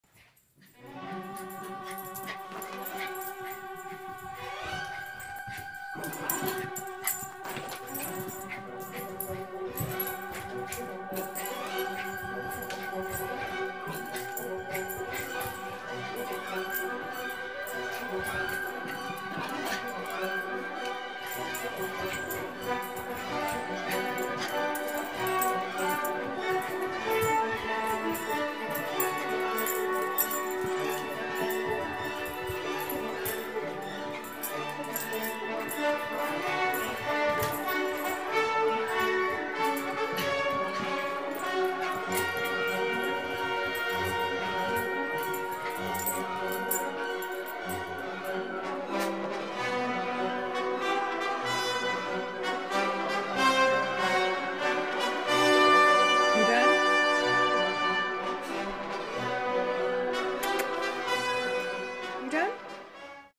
The Usain Bolt of Poodles!!
This is my miniature poodle Cupcake. She enjoys long walks in the beach and running laps around the house as if she were an Olympian!